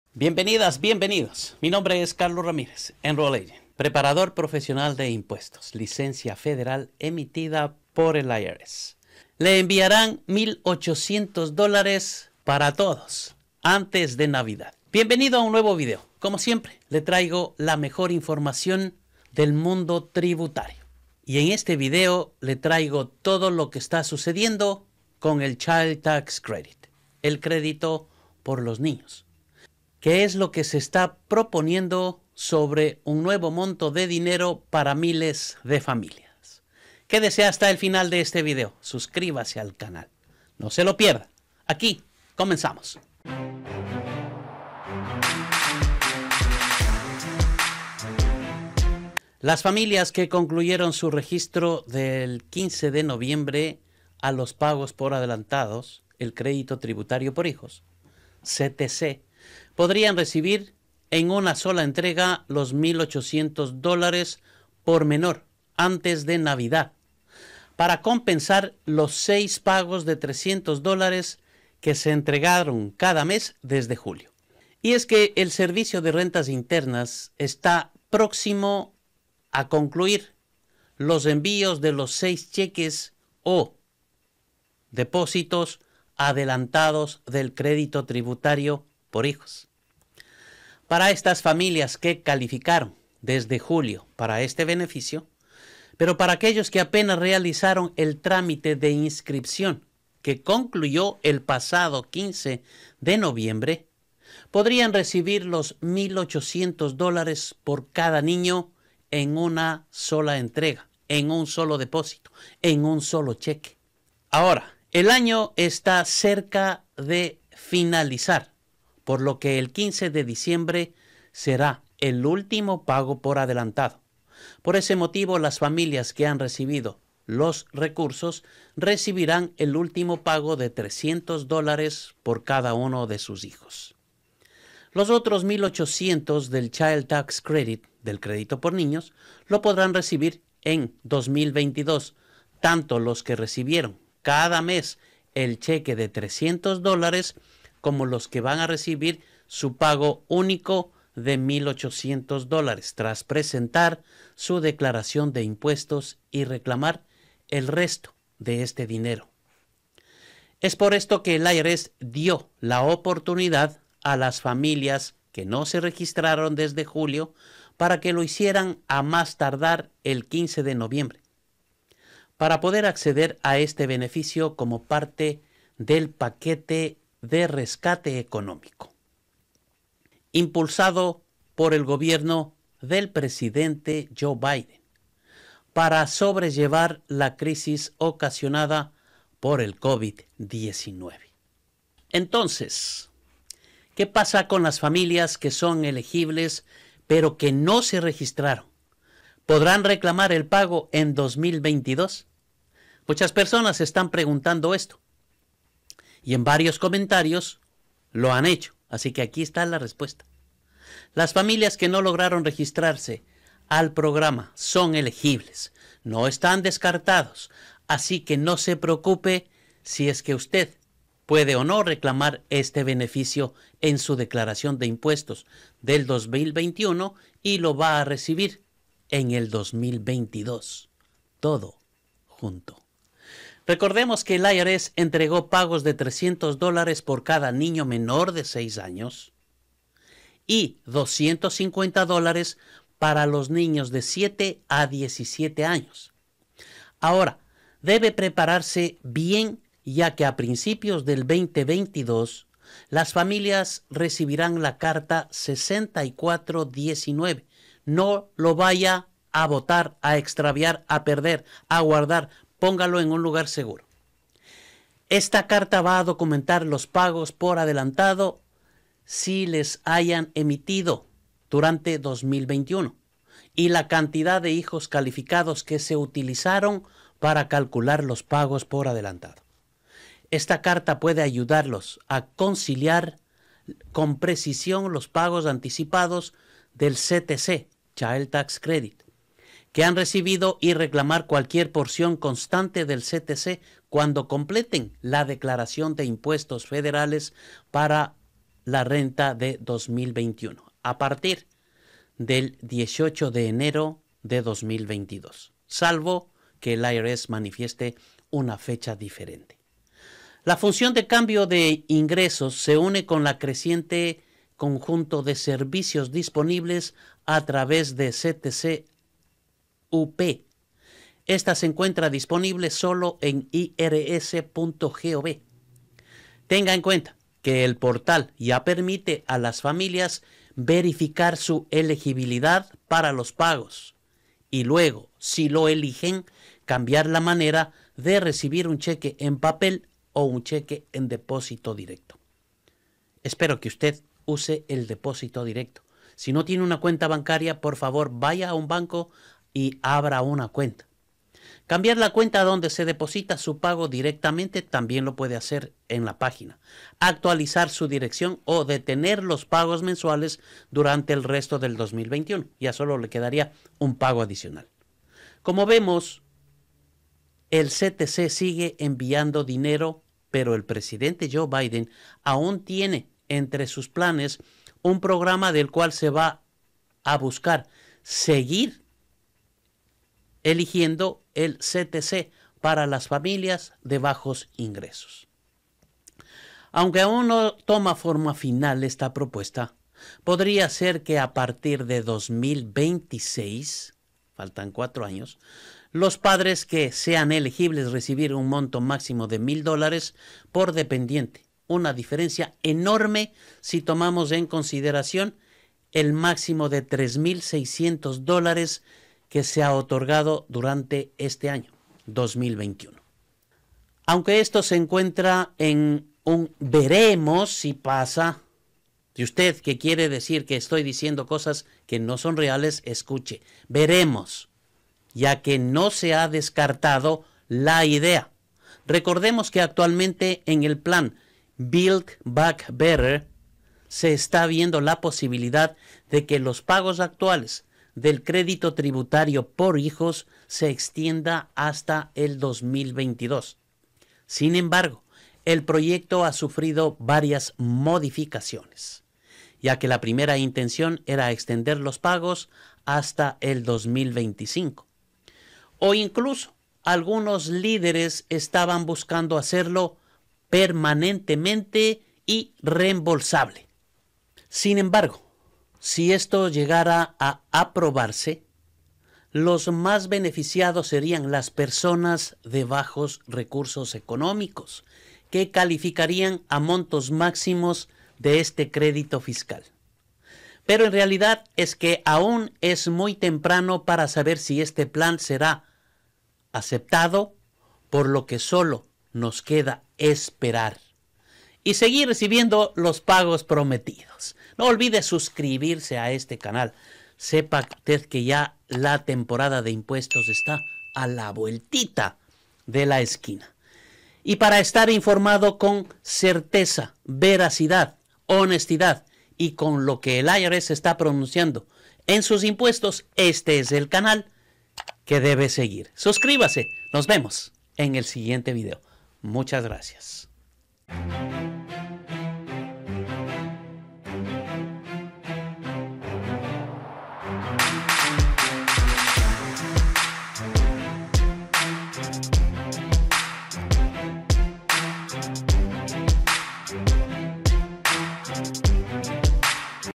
El CTC continúa Actualizándose para AYUDAR A MILES de FAMILIAS | Impuestos en USA
En este video le traigo todo lo que está sucediendo con el Child Tax Credit -CTC y qué es lo que se está proponiendo sobre un nuevo monto de dinero para miles de familias. No se lo pierda. ¡Bienvenidos!

MI PÁGINA OFICIAL - CARLOS RAMIREZ EN FACEBOOK
SÍGAME EN FACEBOOK Y ENTERESE DE TODO